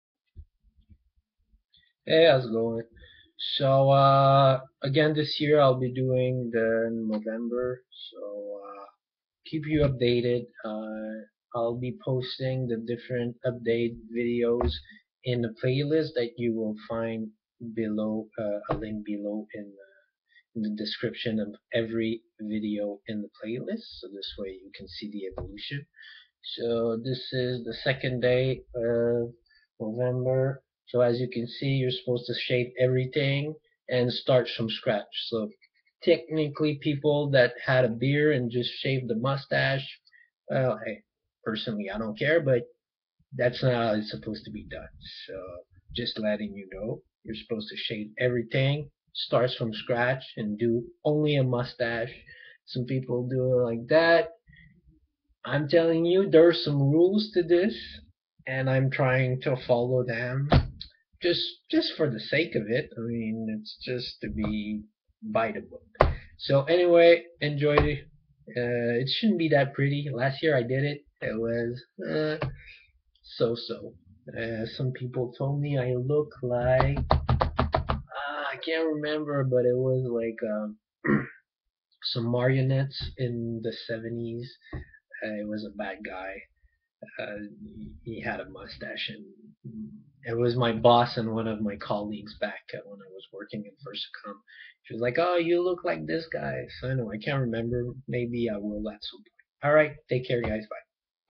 Movember 2013
In Solidarity to my friend and neighbor Claude Rivard, currently battling prostate cancer.

It's a start.

Again this year I will be doing the movember challenge, follow my updates in the following playlist to see the evolution of the moustache